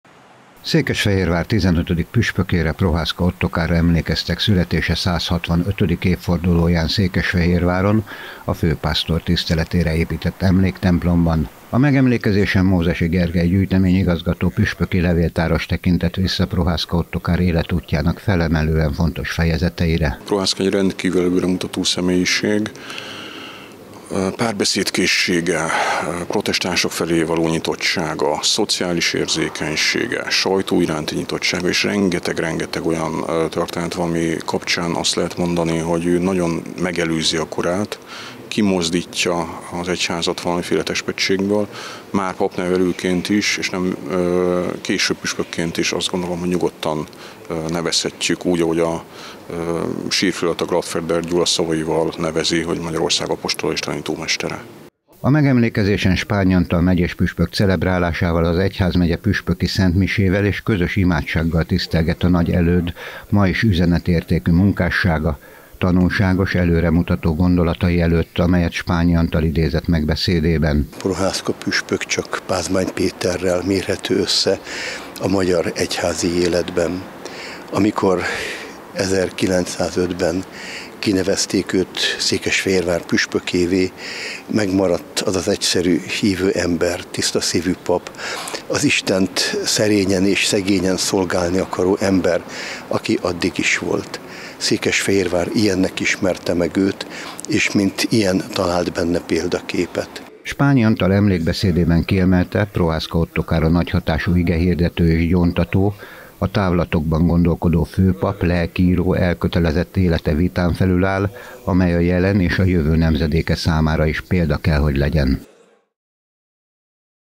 PROHÁSZKA OTTOKÁRRA EMLÉKEZTEK
Prohászka Ottokár püspökre emlékeztek születésének 165. évfordulóján. A Főpásztor emlékmiséjén Mózessy Gergely tekintett vissza életútjára, majd Spányi Antal megyéspüspök idézte meg a főpap emlékét.